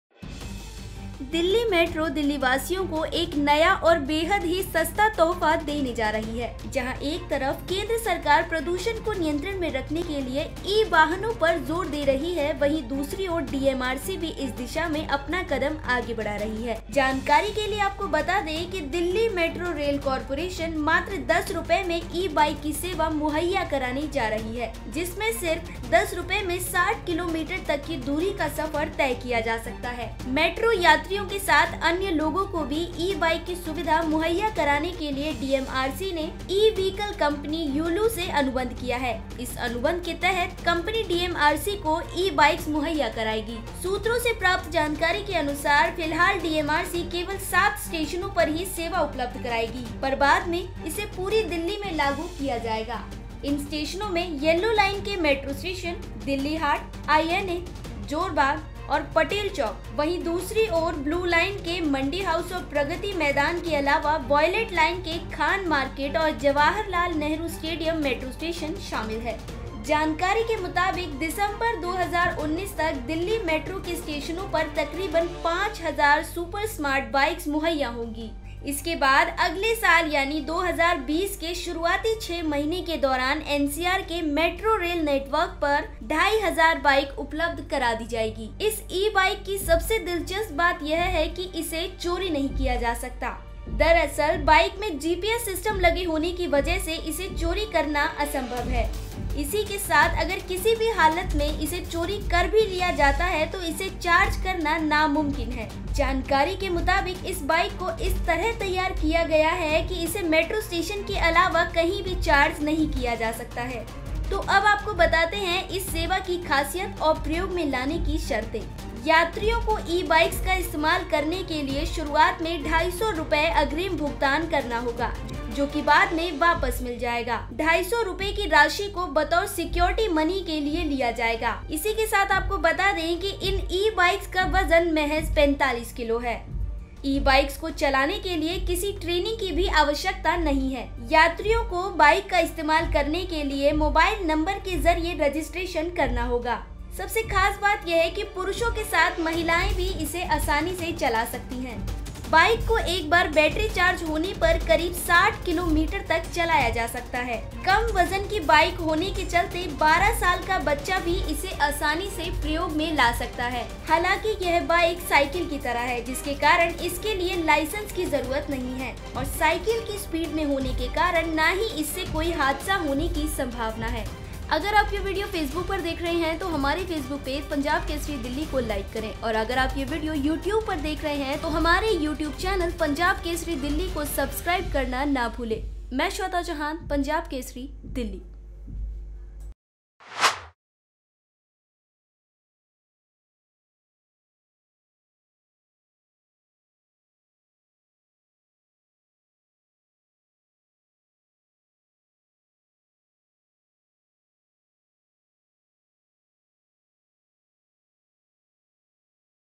DMRC का दिल्ली के लोगों के लिए नया और सस्ता तोहफ़ा | Punjab Kesari Delhi
दिल्ली में बढ़ते प्रदूषण के चलते जहां केंद्र में सत्तासीन नरेंद्र मोदी सरकार (Narendra Modi Government) की ओर से इलेक्ट्रिक वाहनों को बढ़ावा दिया जा रहा है, वही इस दिशा में दिल्ली मेट्रो रेल निगम (Delhi Metro Rail Corporation) ने भी अहम कदम उठाया है। इसके तहत डीएमआरसी यानी दिल्ली मेट्रो (Delhi Metro) मेट्रो स्टेशनों पर सिर्फ 10 रुपये में ई-बाइक (Electric bike) की सुविधा देने जा रहा है। सिर्फ 10 रुपये देकर इन ई-बाइक्स से 60 किलोमीटर की दूरी तक सफर तय किया जा सकेगा।